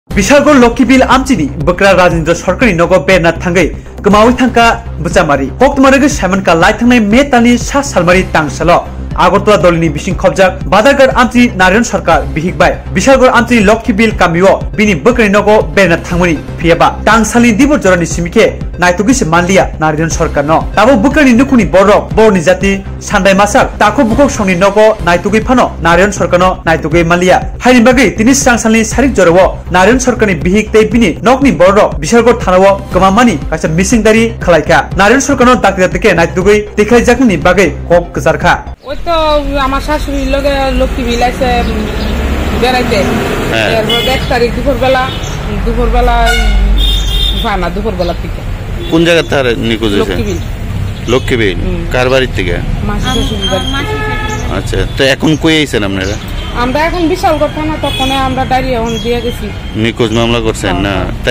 Bwkrabari Beraina Phaiwi Kwmajak Bwchamari
Husband & wife went to their  father-in-laws house husband got missing while out for marketing an FIR has been logged at Bishalgarh Police Station